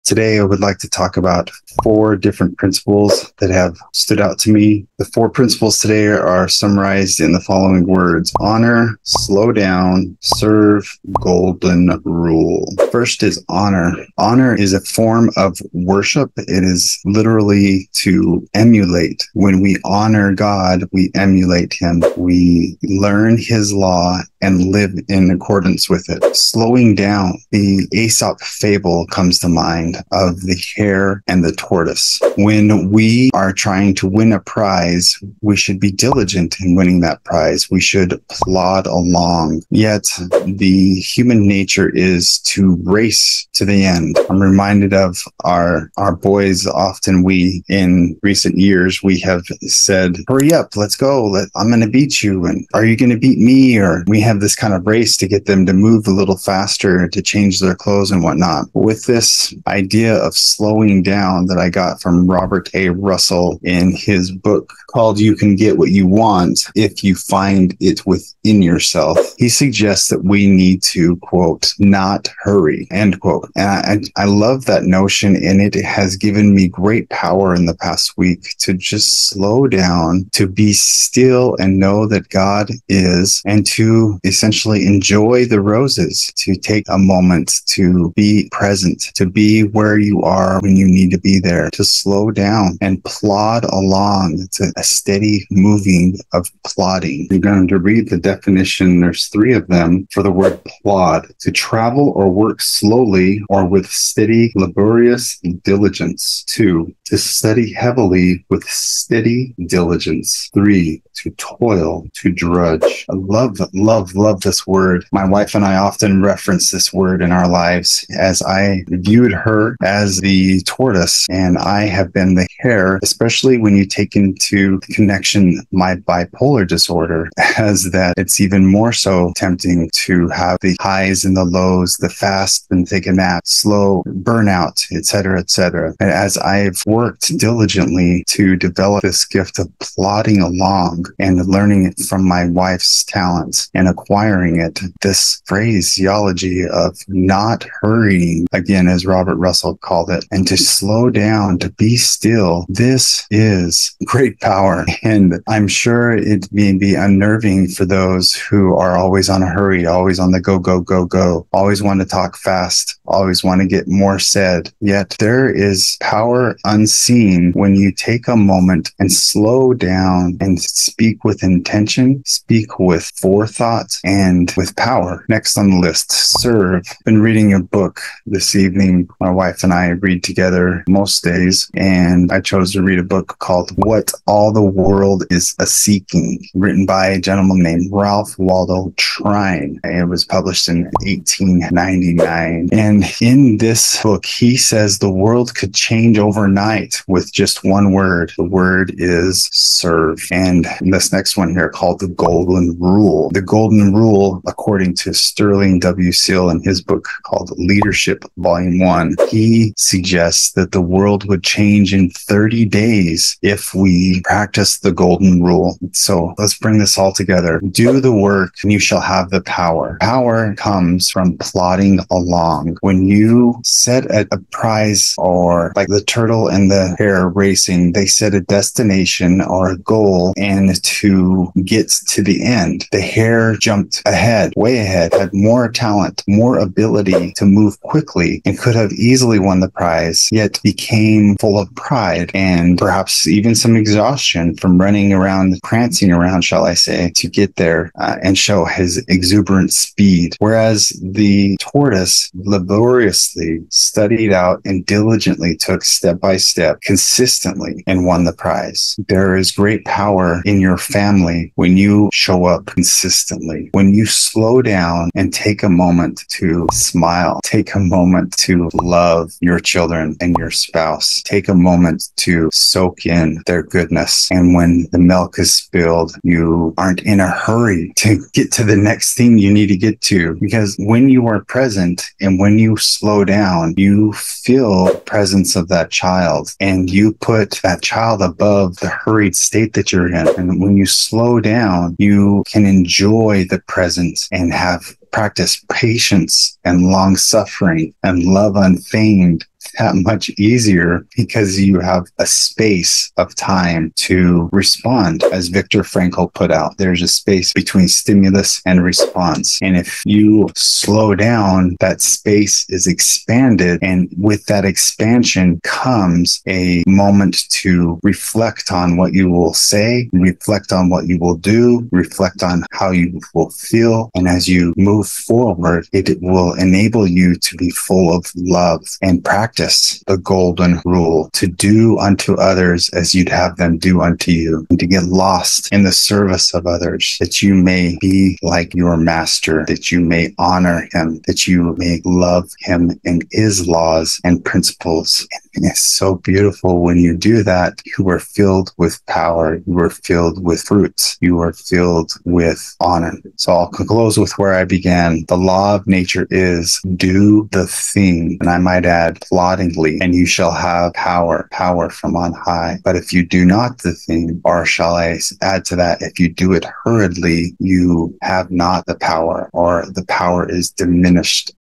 Slow ... down.
“The law of nature is, Do the thing, and you shall have the power: but they who do not the thing have not the power."
Essays | Compensation | Ralph Waldo Emerson

Kent shares how to access more power and patience as a Father. The following four ideas are elaborated on:
1. Honor
2. Slow down
3. Serve
4. The golden rule

Works referenced:
Robert A. Russell's "You can Get What you Want if You find it Within Yourself"
Ralph Waldo Trine's "What all the World's a Seeking"
Sterling W. Sill's "Leadership, Vol. 1"
Viktor Frankl “Man’s search for Meaning“